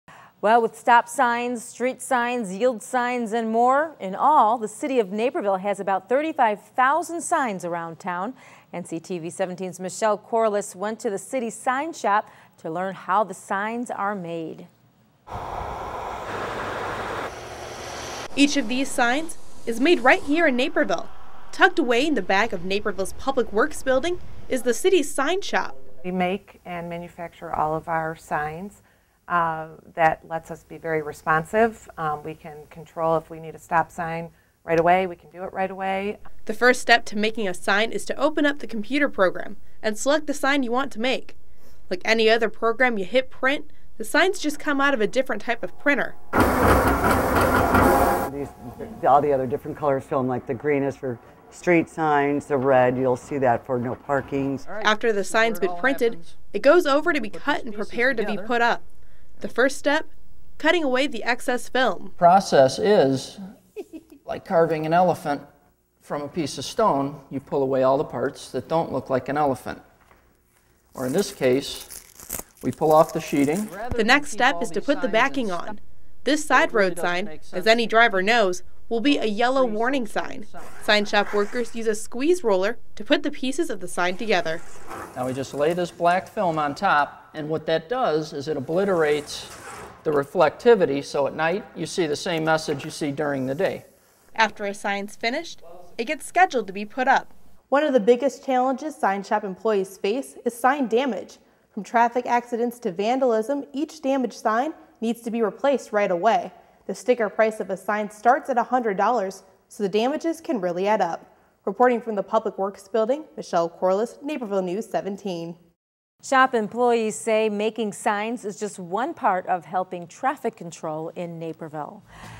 One Stop Shop
With stop signs, street signs, yield signs and more, in all, the City of Naperville has about 35,000 signs around town.  NCTV17's Michelle Corless went to the city sign shop to learn how the signs are made.